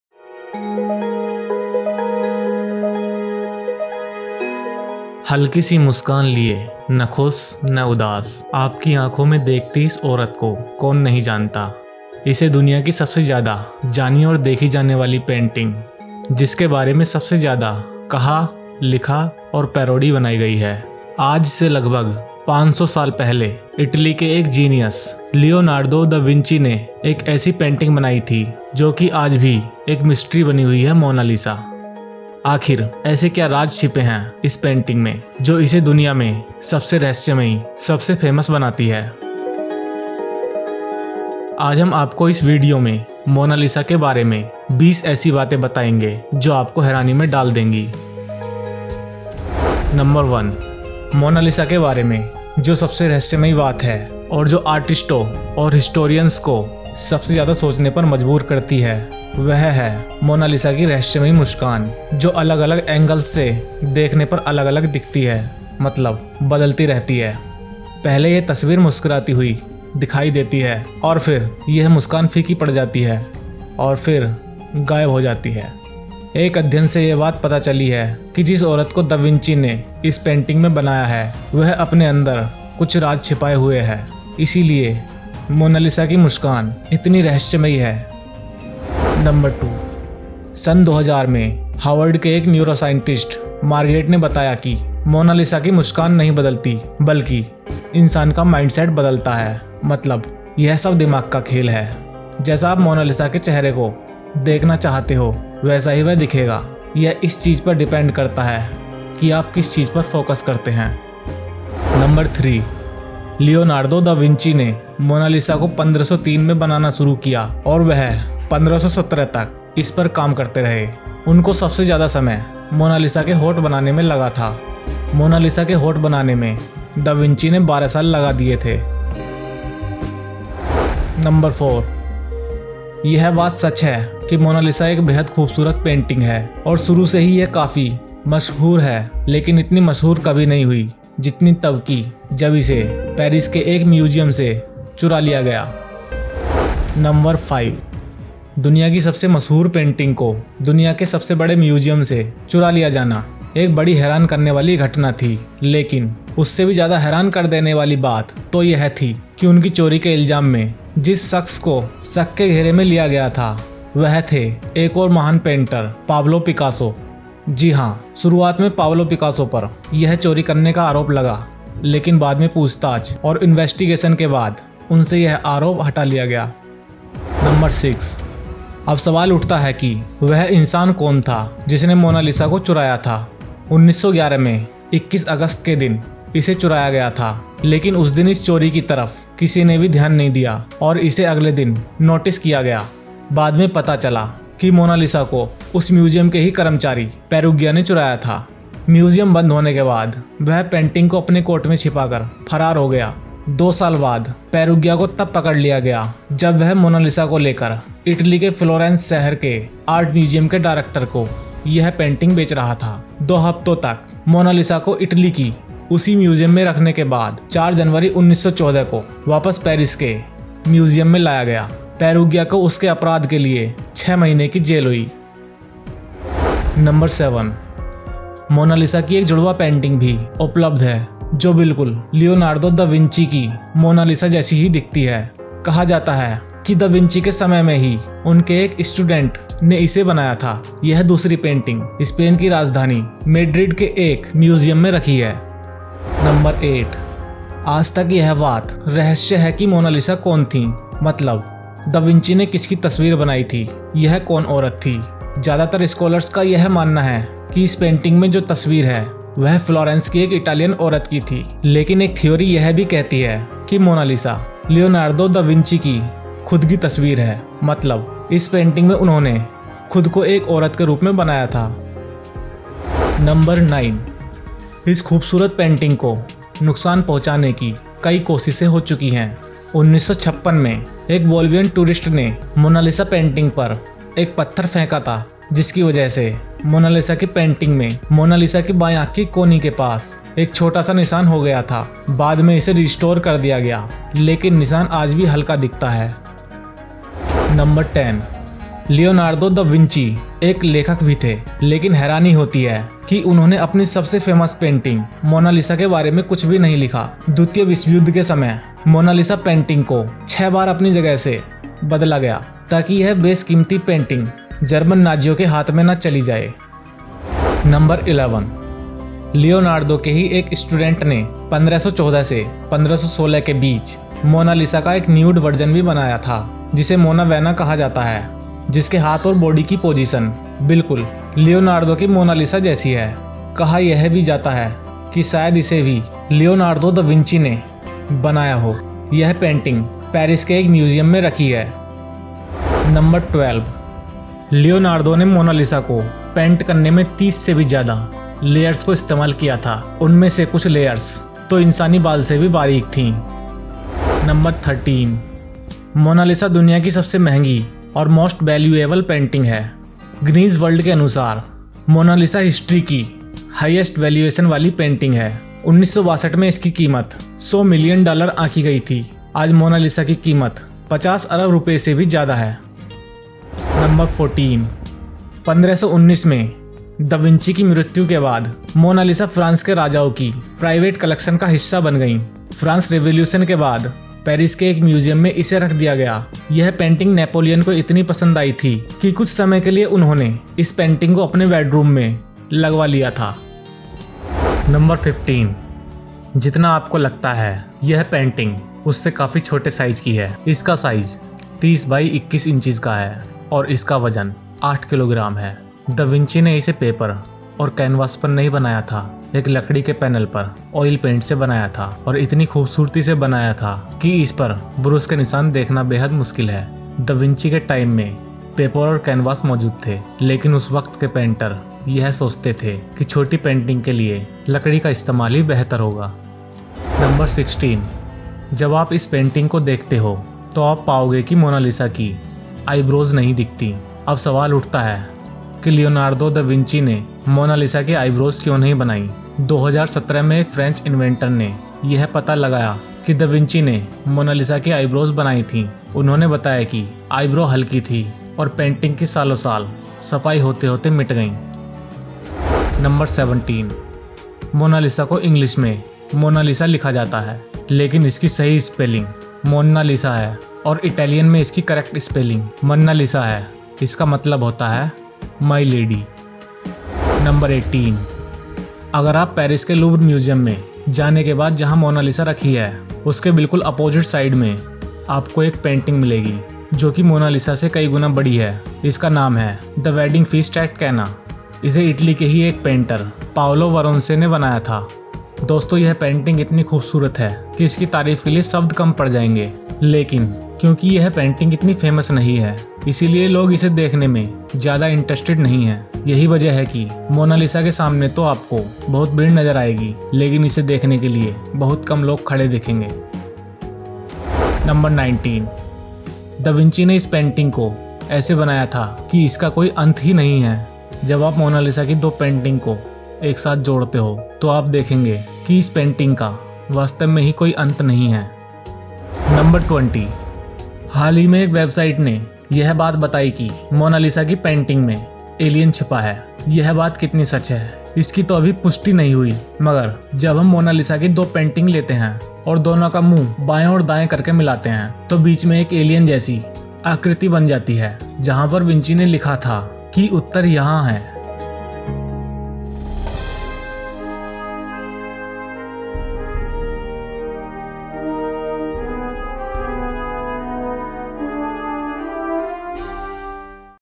मोनालिसा का इतिहास और तथ्य हिंदी... Monalisa History and Facts in Hindi
500 साल पहले इटली के एक जीनियस लियोनार्दो दा विंची ने एक ऐसी पेंटिंग बनाई थी जो कि आज भी एक मिस्ट्री बनी हुई है “मोनालिसा”
500 years ago Leonardo da Vinci, a genius from Italy, created a painting that still remains a mystery "Monalisa"